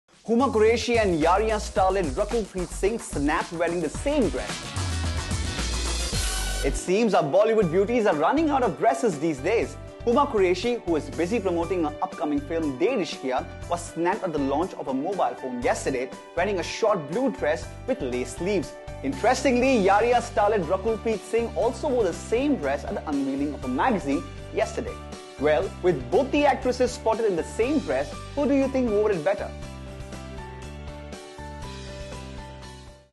Huma Qureshi & Rakul Preet Singh wearing same Outfit
Huma Qureshi & Rakul Preet Singh wearing same dress

Both the B-town babes were spotted wearing the same dress at different events.
In an personel intweview, both the actors spoke about their dress while none of them were aware about eachother wearing the same dress.